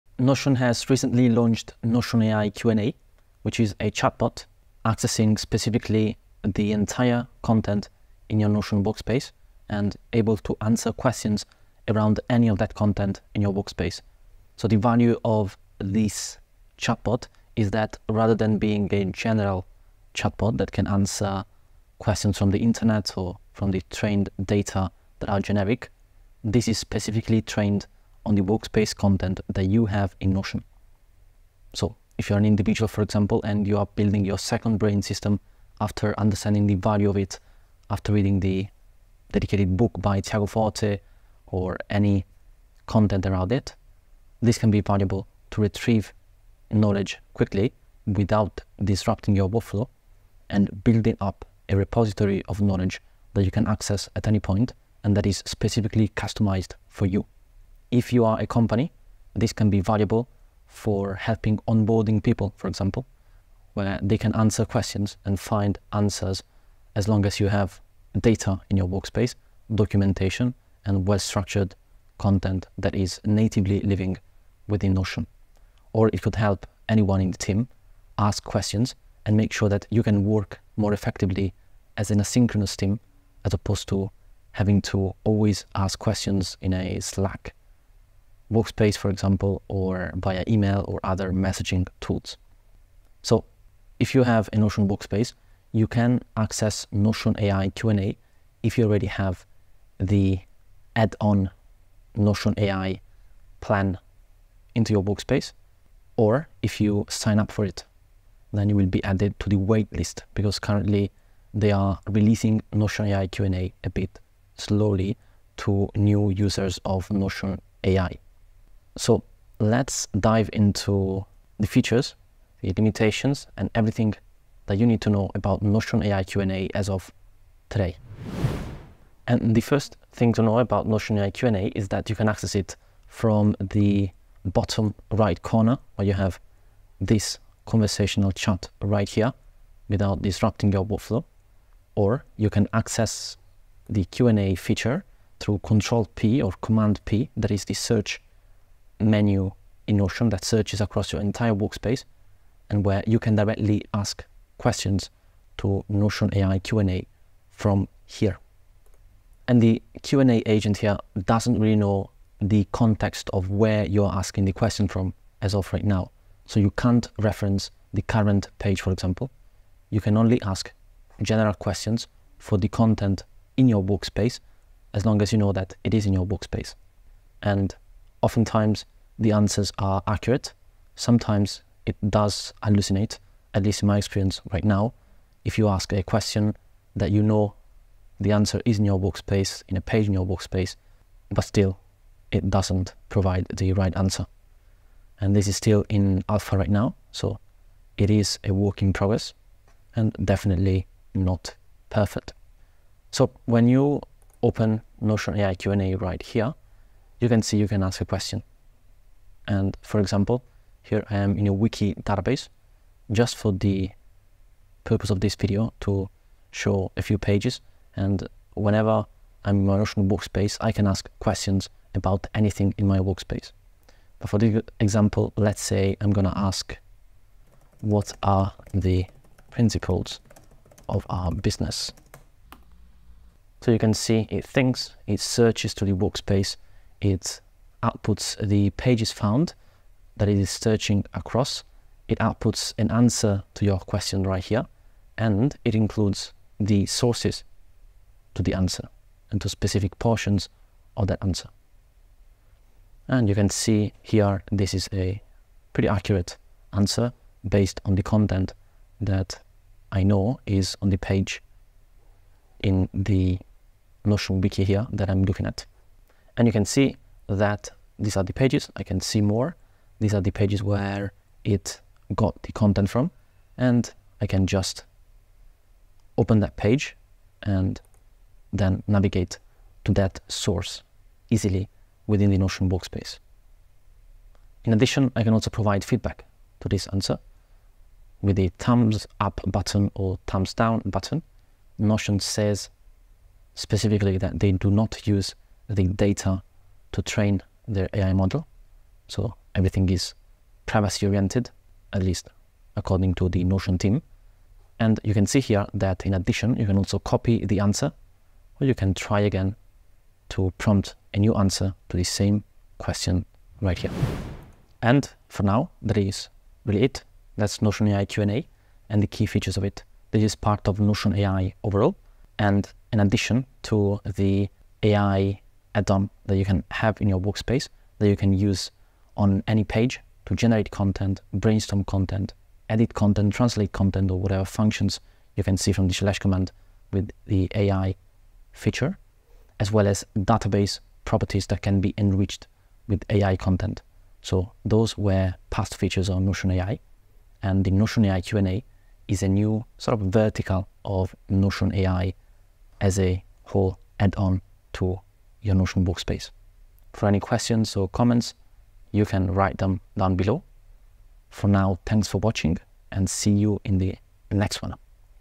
Exploring Notion AI Q&A: Your New Workspace Companion - Guide to Notion AI Q&A
- Get up to $1,000 off on a Notion team plan (for startups)

TIMESTAMPS
0:00 Introduction
1:33 How to get access
2:10 How to access Notion AI Q&A
3:25 Testing Notion AI Q&A
5:15 Conclusion